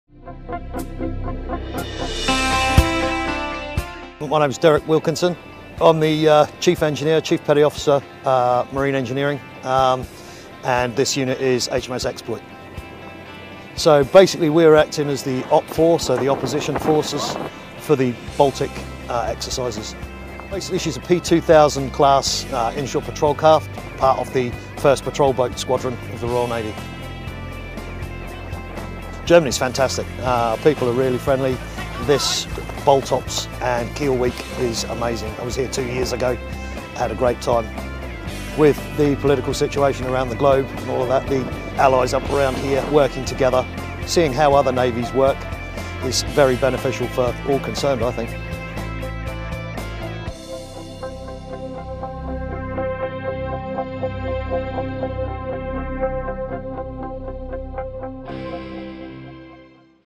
Royal Navy Chief Petty Officers Talks about BALTOPS 2019
Chief Petty Officer Derek Wilkinson, assigned to the Royal navy Archer-class patrol vessel HMS Exploit (P167), represents Faces of BALTOPS during exercise Baltic Operations (BALTOPS) 2019. BALTOPS is the premier annual maritime-focused exercise in the Baltic Region, marking the 47th year of one of the largest exercises in Northern Europe enhancing flexibility and interoperability among allied and partner nations. (U.S. Navy video by Mass Communication Specialist 2nd Class Jessica L. Dowell)